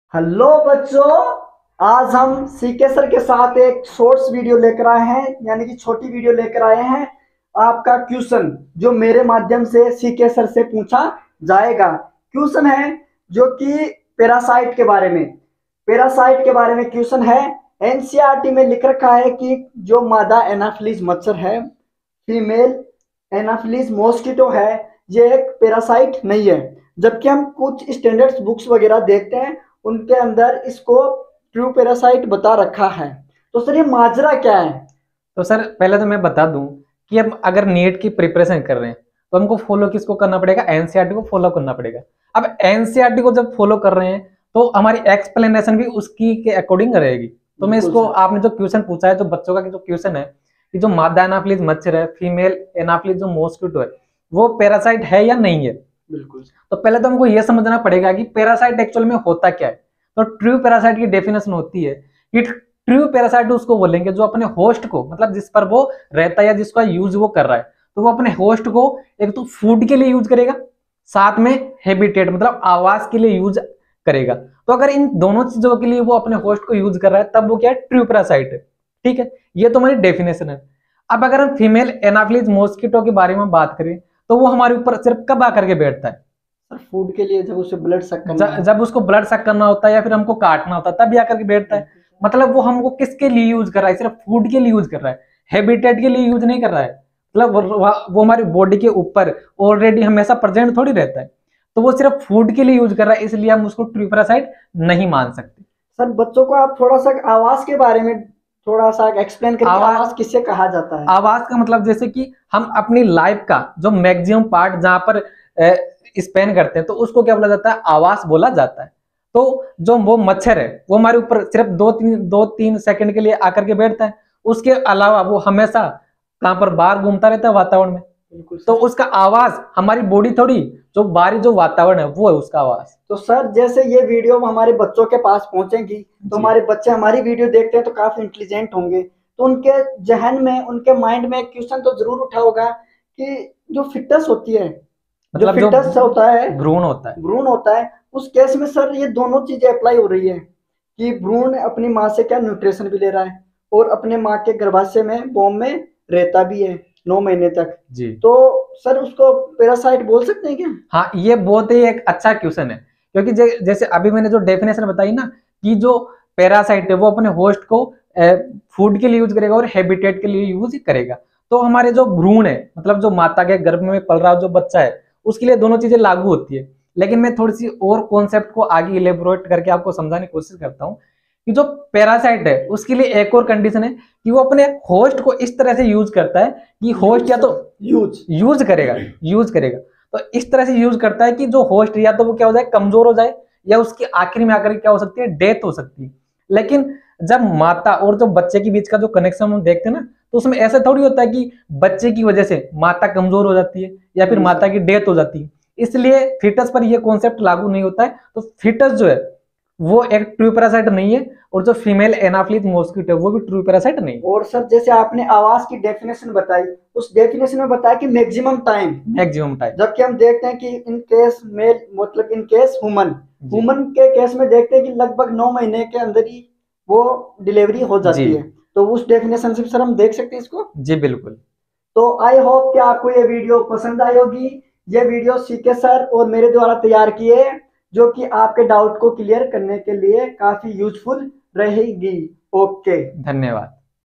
Female Anaphalis Mosquito is parasite or not
what is parasite
what is true parasite
what is false parasite
why female anaphalis Mosquito is not true parasite